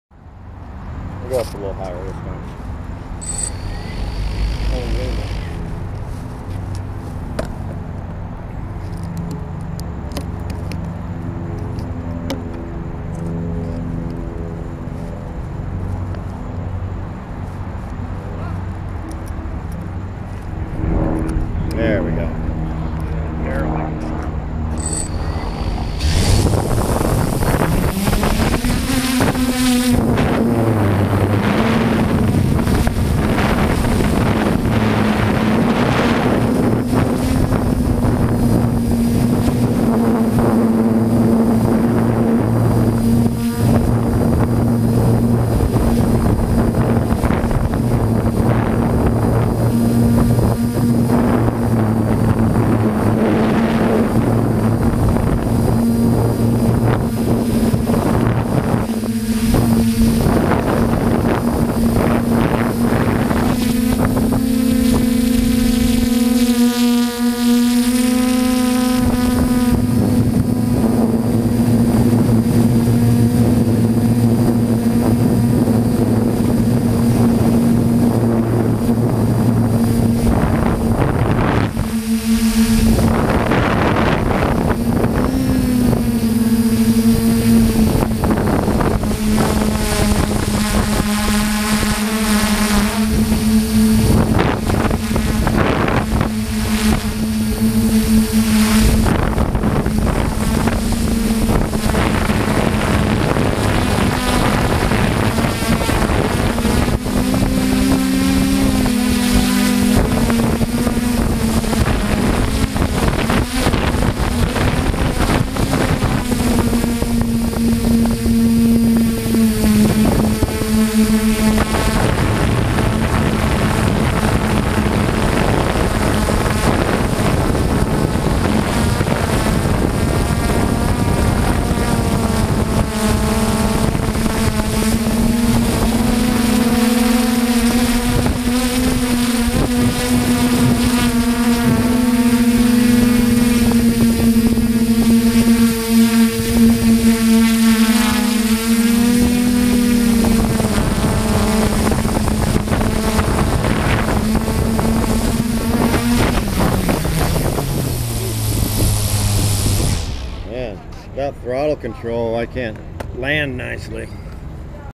New York by air
This is my first test flight with a gimbal on my Flame wheel 450.
It's very over weight with dji motors. A motor change and maybe 4s batteries and another test flight.